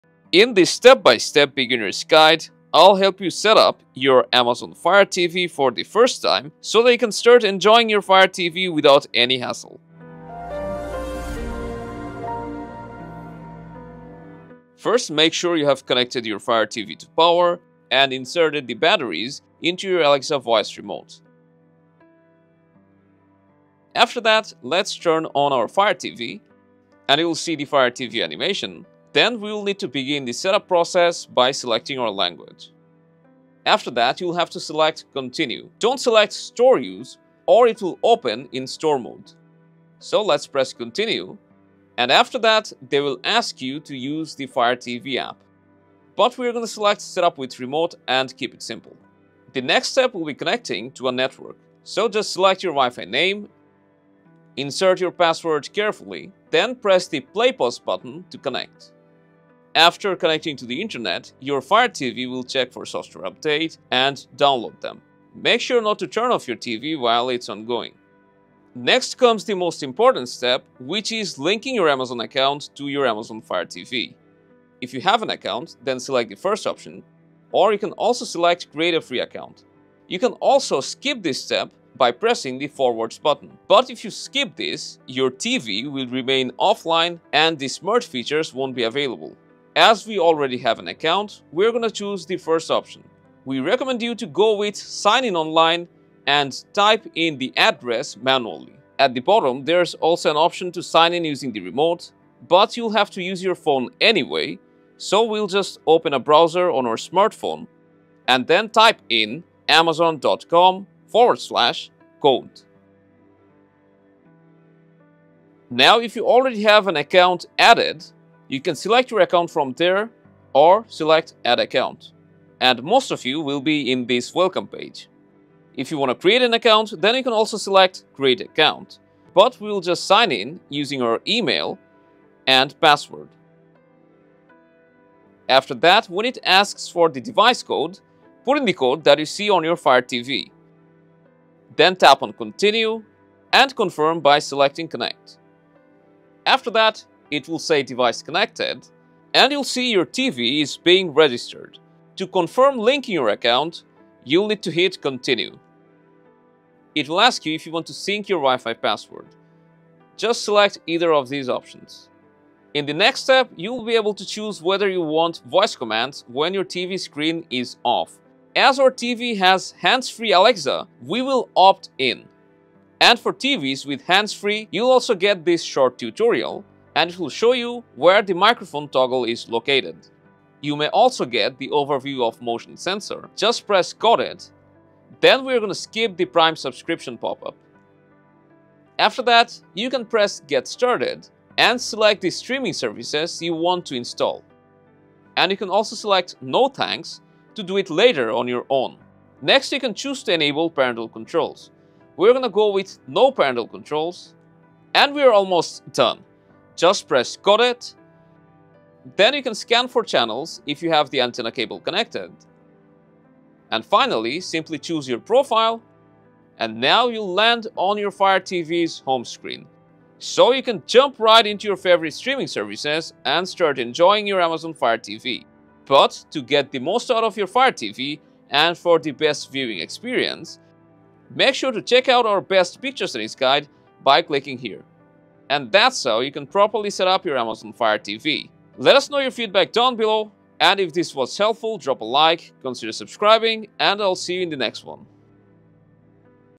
Amazon Fire TV: How to Setup For The First Time! [Beginners Guide]
Struggling with the initial setup of your new Amazon 4K Fire Television with Fire OS after just getting it for the first time? Looking for a step-by-step guide to properly configure your Amazon Fire TV as a new user? Well, you’ve come to the right video.

The Fix369 Team will walk you through a 3-step beginner’s guide to set up your Amazon 4K Fire TV for the first time. We will also show you how to link your Amazon Account and enable hands-free Alexa voice command on Amazon Fire TV 4K as well.

0:00 Intro: Amazon Fire TV First Time Setup
0:15 Step 1: Power ON and Initial Setup
1:11 Step 2: Link your Amazon account
2:45 Step 3: Set up Voice Command and complete the setup